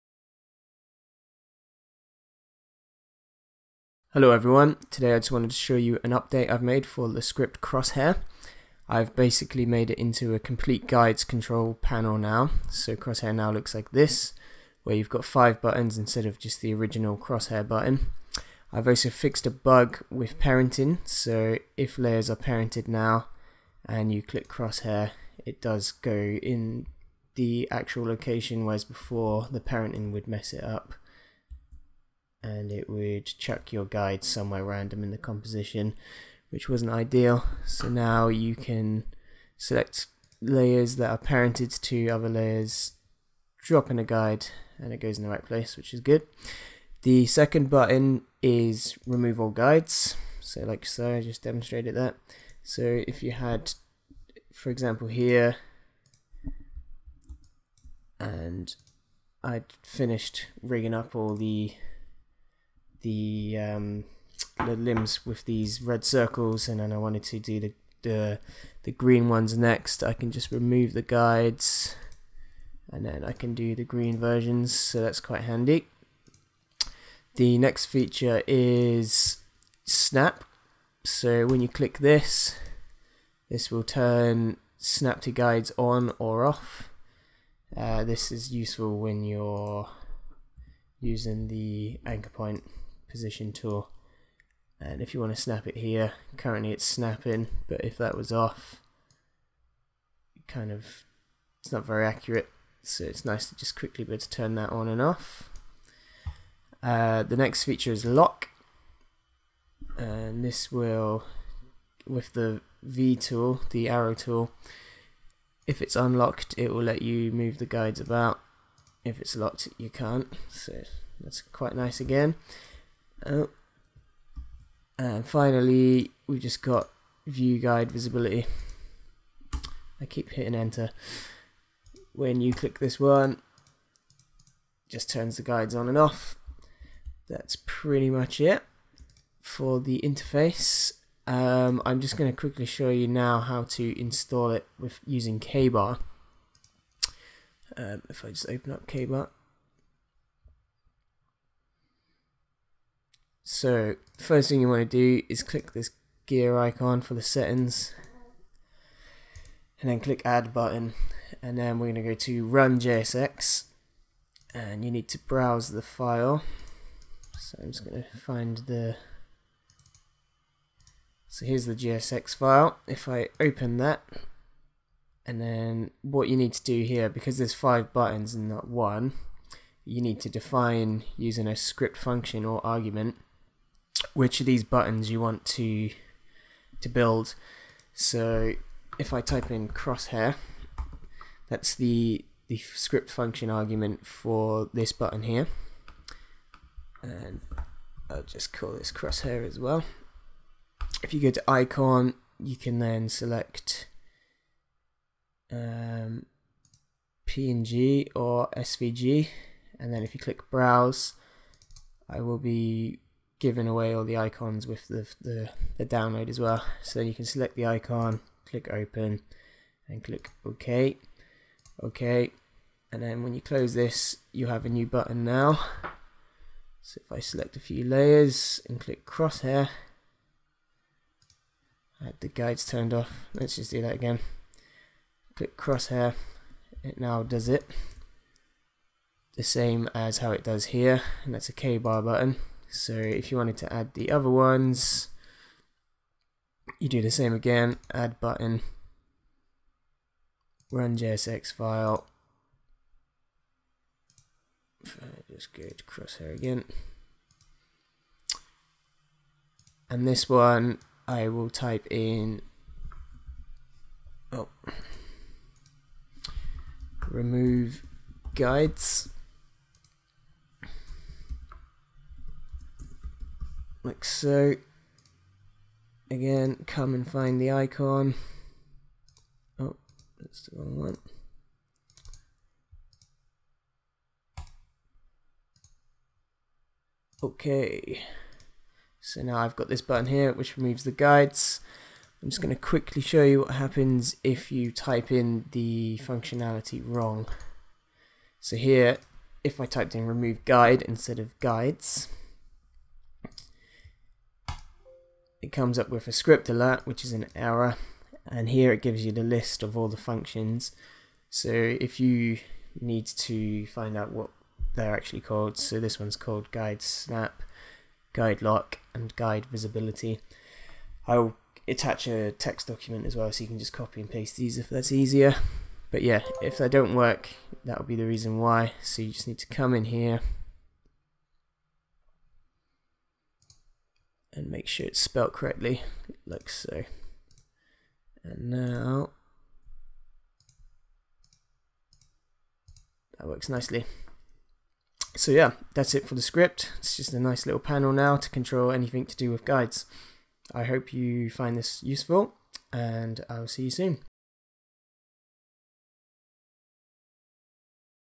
After Effects Script Demo - Crosshair Update 1.1
Crosshair is a Guide Manager Tool for After Effects 2019 onwards. Crosshair gives you the following five buttons:

Crosshair - Creates X and Y Guides for selected layers positions.

Remove Guides - Removes all Guides from a Viewer.

Guide Snap - Toggles Guide Snapping.

Guide Lock - Toggles Guide Locking.

Guide Visibility - Toggles Guide Visibility.

Crosshair was built to help speed up the rigging process when moving layer Anchor Points or positioning DUIK bones and Rubberhose or Limber start and end controllers.